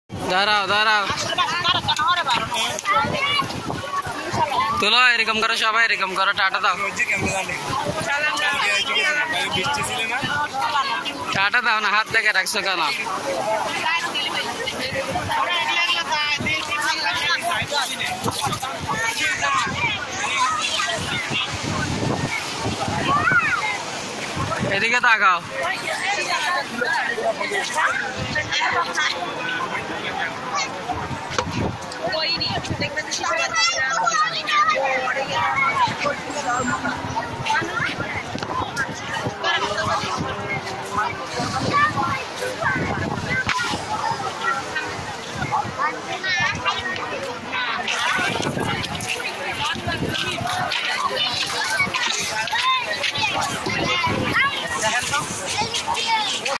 Potenga sea beach | All of my nieces at sea beach
Potenga sagor par, সাগর পাড়ে অনেক মজা।